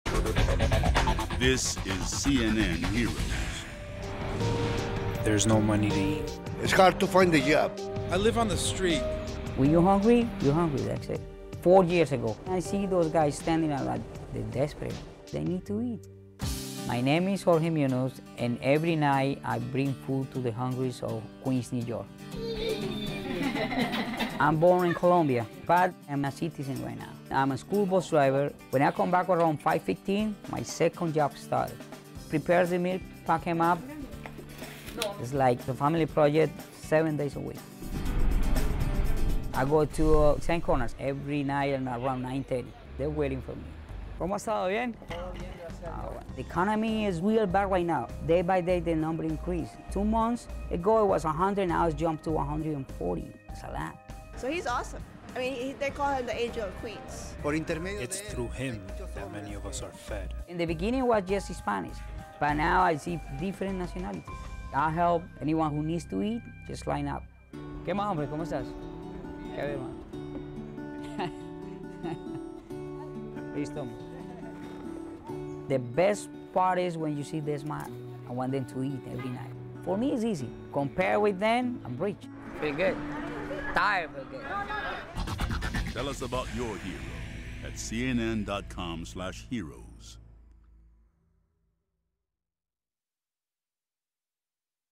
Bus driver delivers free home cooked meals - CNN Hero
Every night, Jorge Munoz brings food to unemployed men in Queens, New York.

Munoz estimates he's served more than 70,000 meals since 2004.

He and his family fund operation through their savings and his weekly $700 paycheck.